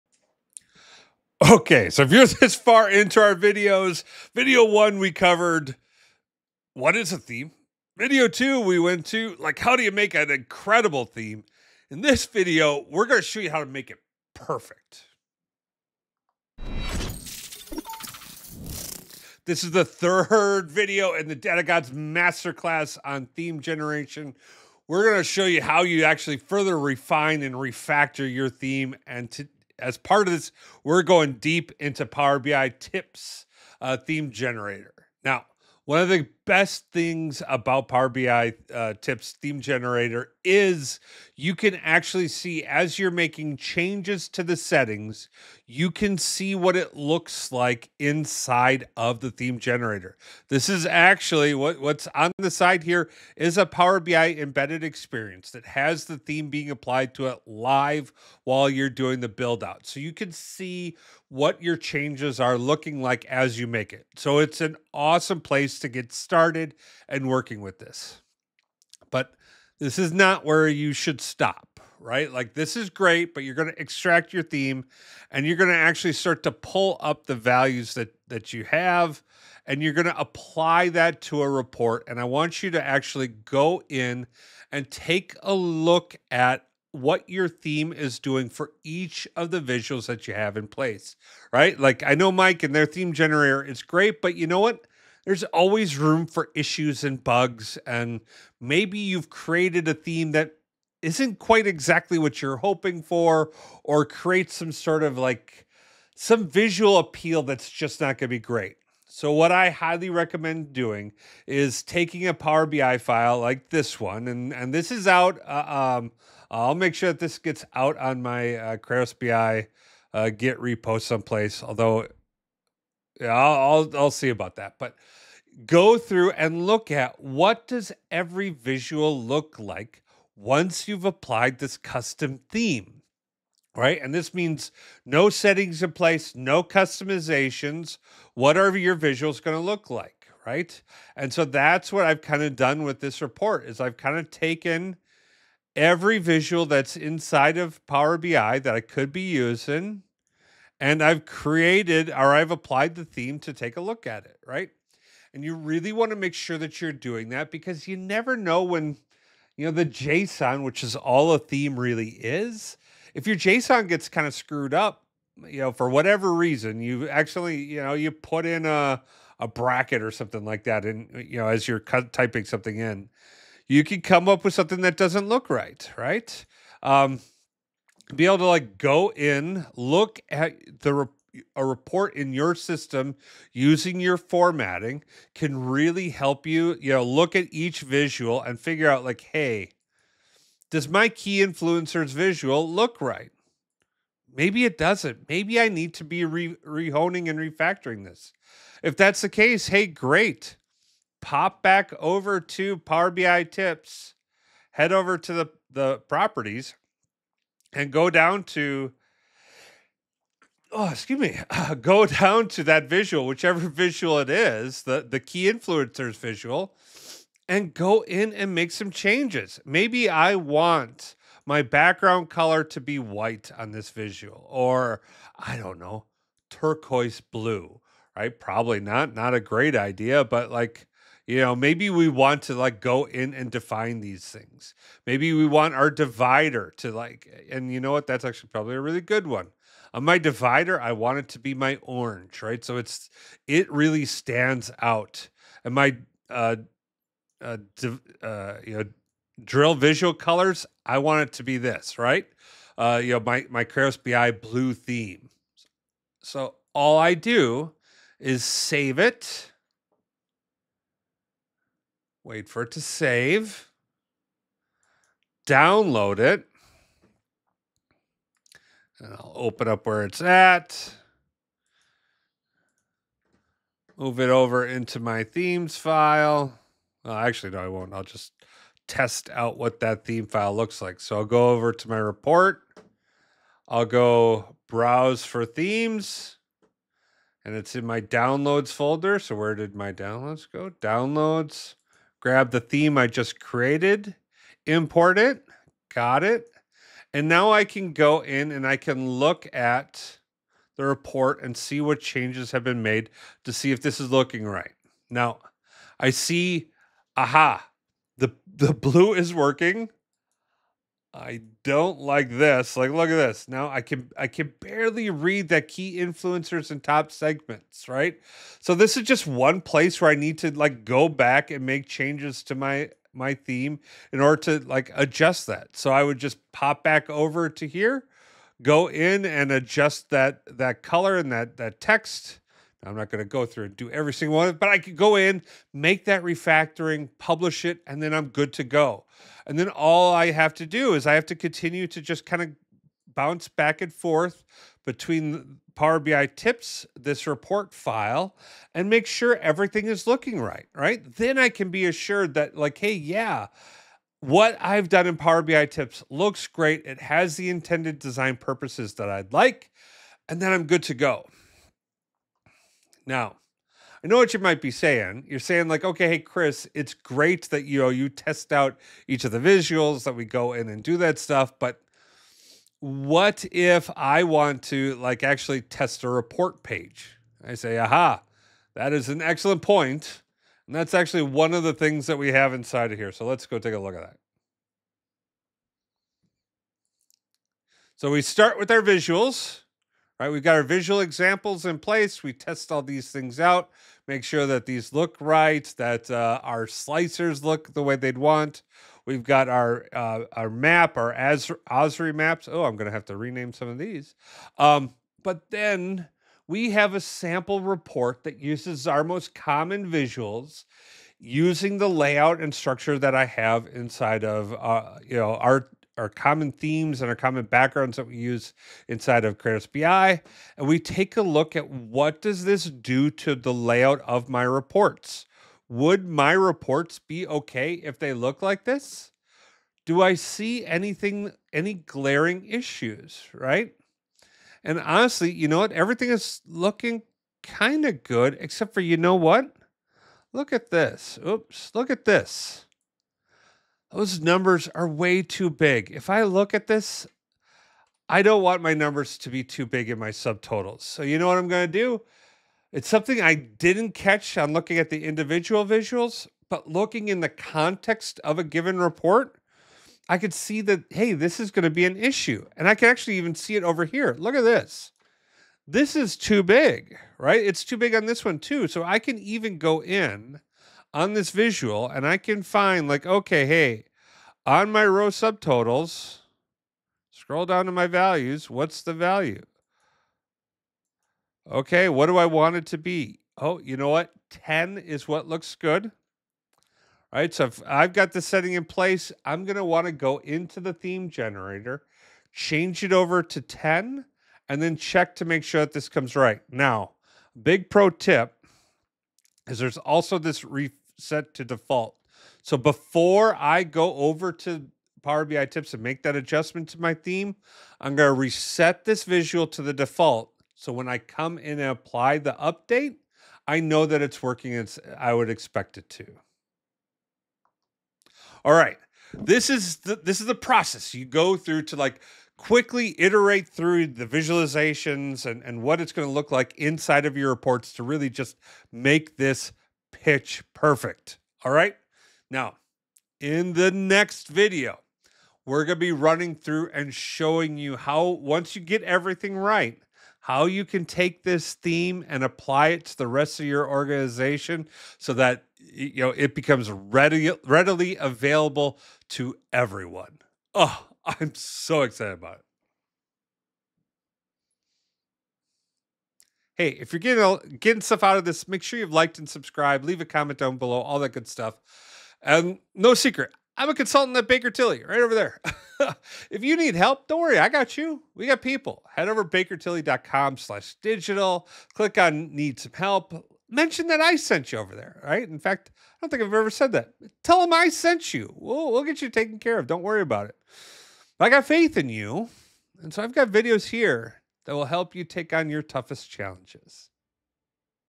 Perfecting Your Power BI Theme | Validation & Iteration Guide (Ep. 3)
Building a Power BI theme is just the start — now it’s time to refine it! In this video, Chris Wagner shows how to test and validate your Power BI theme for color contrast, accessibility, and visual balance.

Learn how to iterate on your design using real dashboards, fix readability issues, and ensure your theme looks great across every Power BI report.